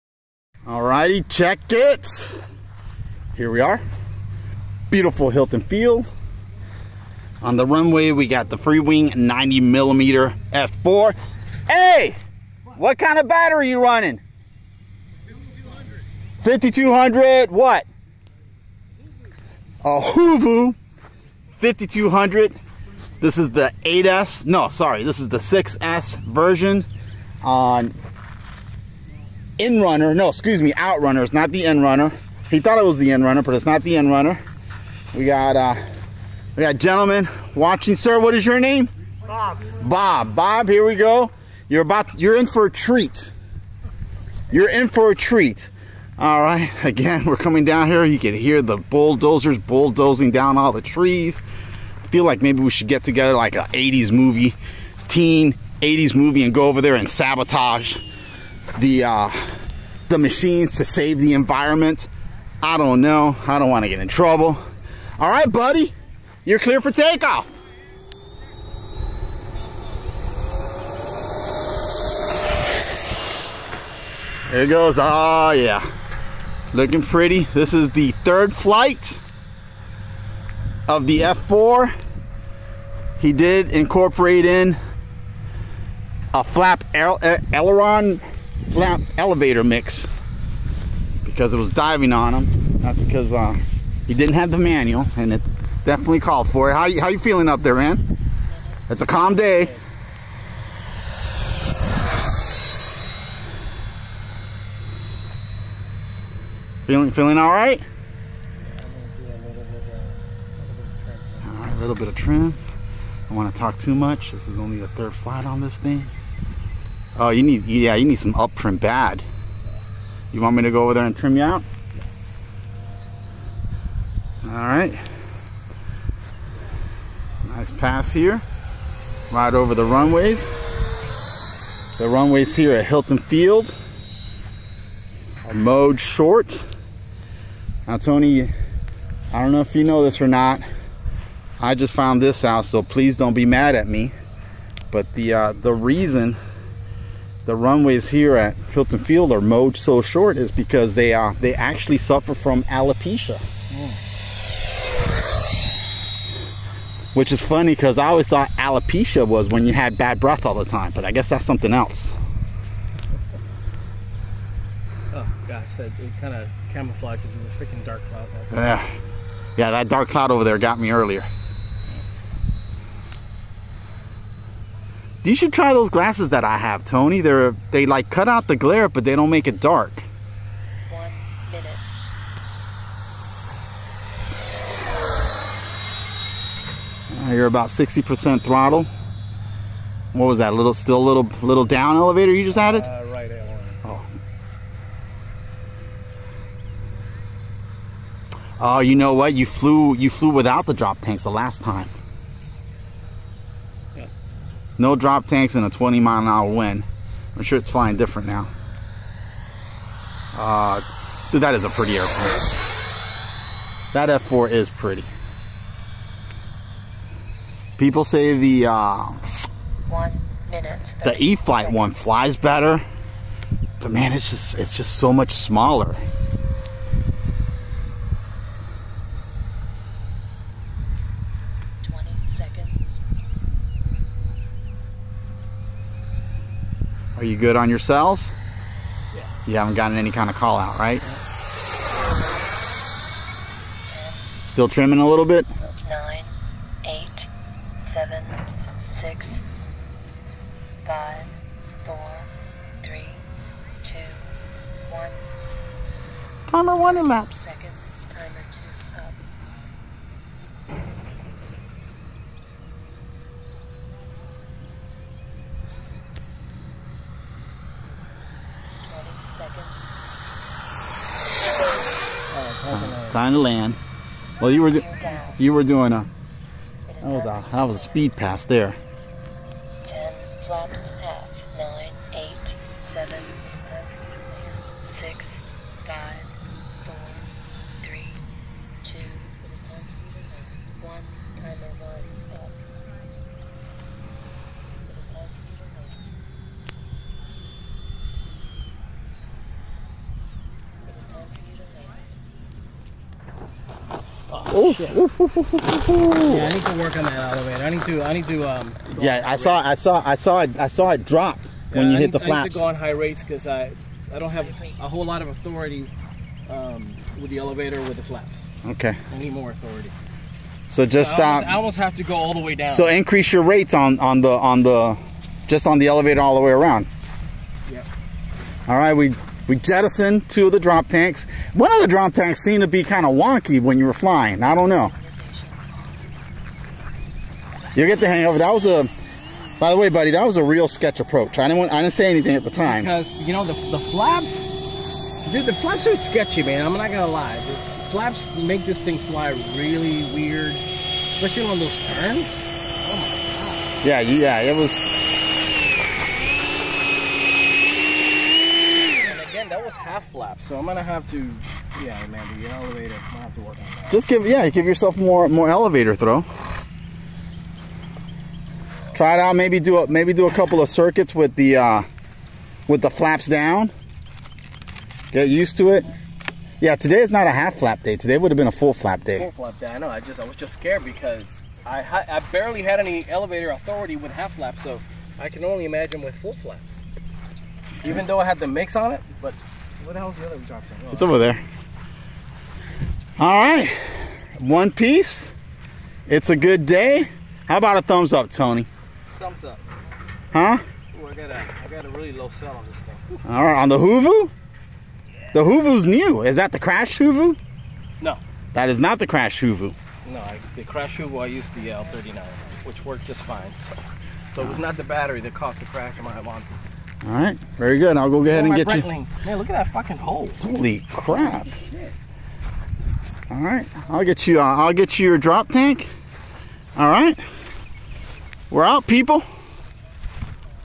F4 Phantom 3rd flight at Hilton Field. Lil Tony not loving it. Approach and landing super sketch!!!
Freewing F4 Phantom on Hoovo 5200 mah 6s. Flaps diminish the elevator authority. Sketchy approach and sketchy landing. the runways suffer form alopecia not halitosis!!!